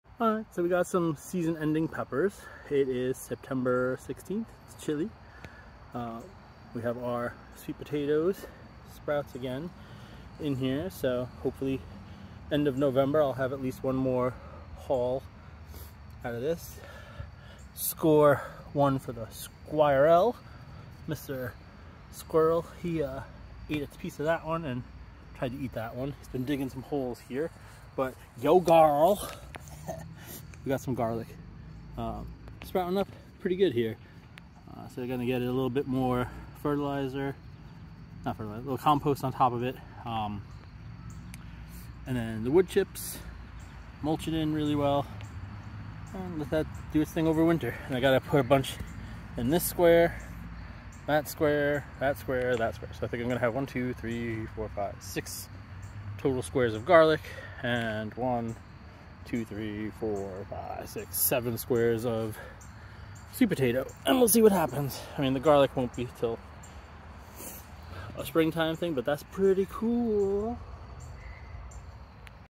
hey Garl😉
Yoooo Garl🤣

Ok but seriously, fall is coming and we are just out here tinkering. I know I'm crossing fingers for a mid  Nov sweet potato harvest from these.. though probably not, can't hurt to try

The sweet potato bags in the grow tent will do fine though. And yo, yooo Garl how you doing. Garlic is doing pretty solid so far. More squares to fill this week and then get it ready for winter.

This one squirrel 🐿️ though keeps visiting daily digging holes but isn't to interested in my garlic 🧄